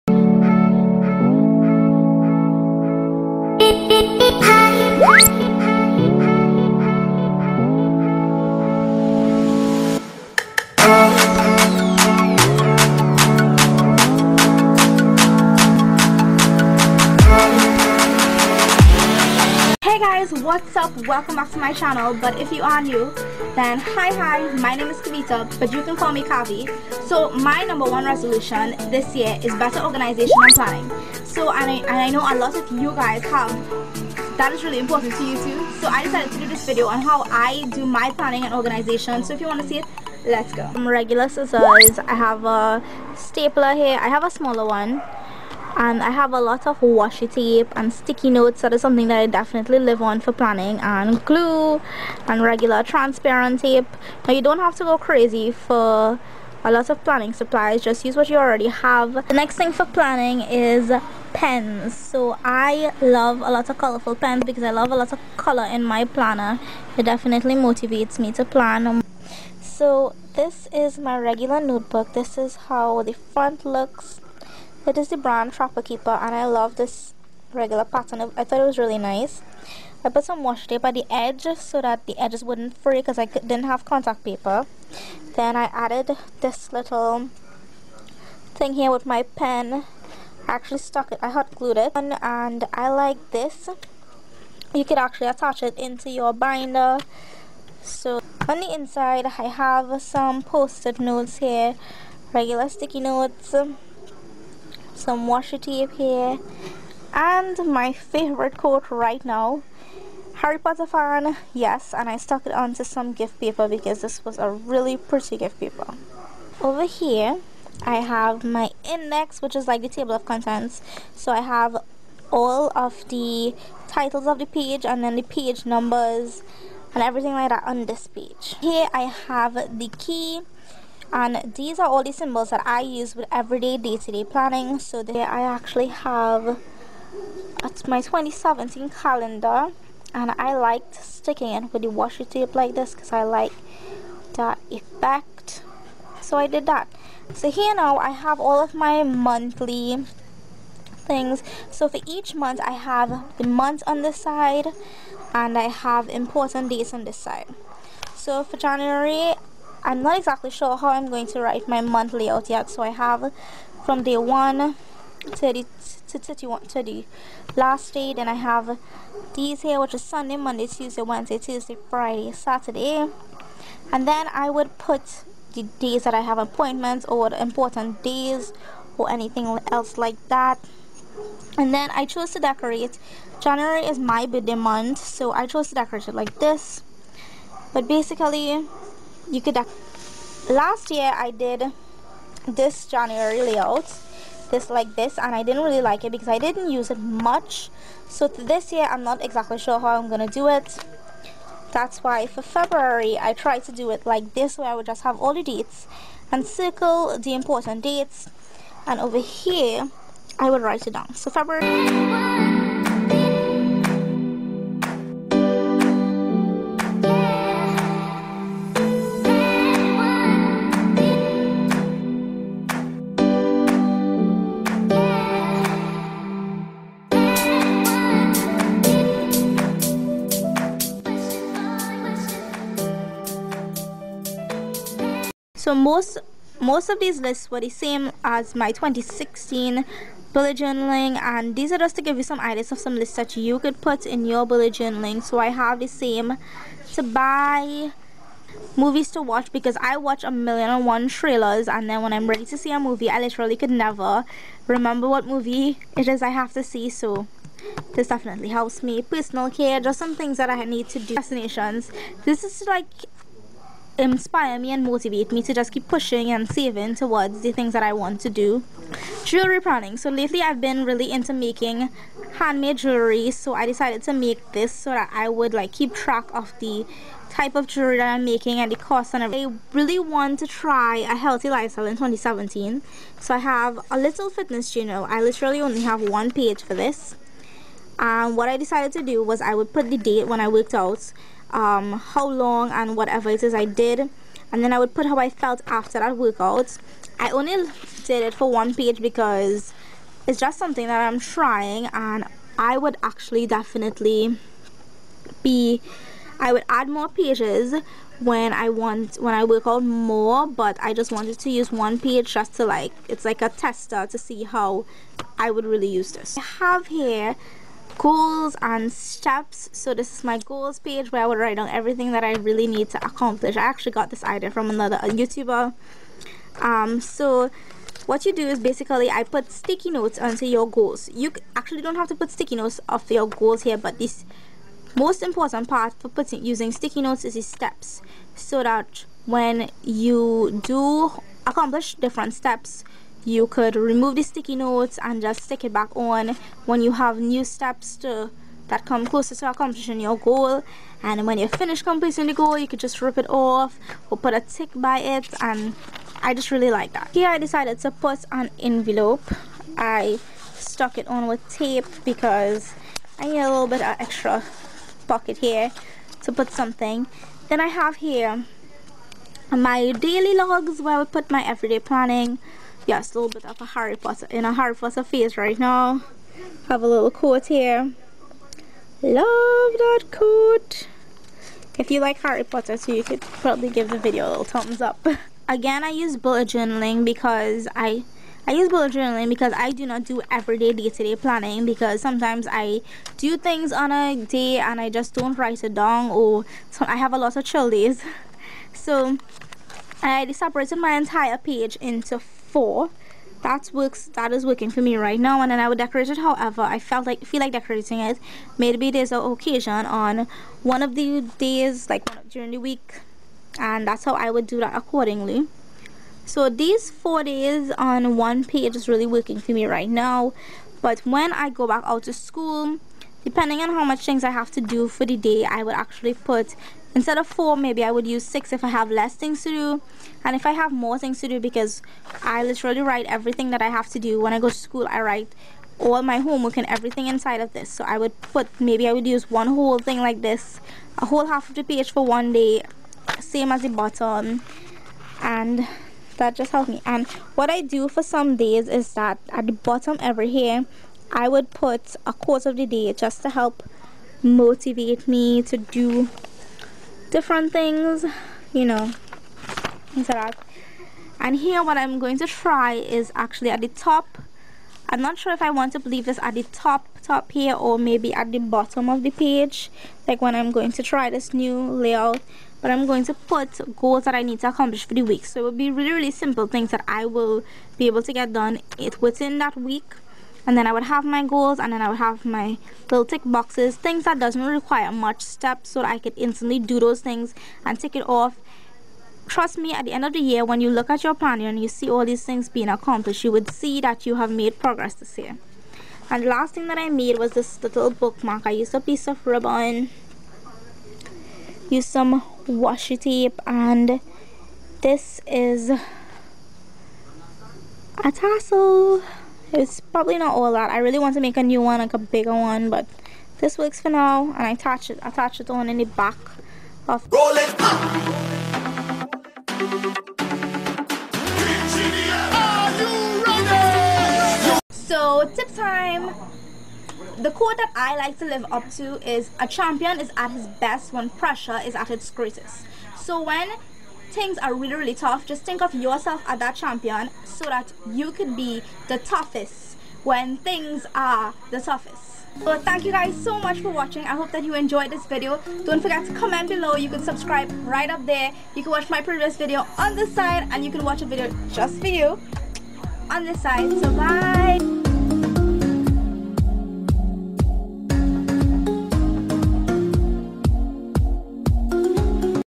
Notebook-Planner/Bullet journal flip through 2017
Hey guys, I am so sorry this video was late. But anyways its all about converting a regular notebook to a planner/ agenda. Its also a bullet journal flip through. Hope you enjoy it. Muahhhh
Check me out: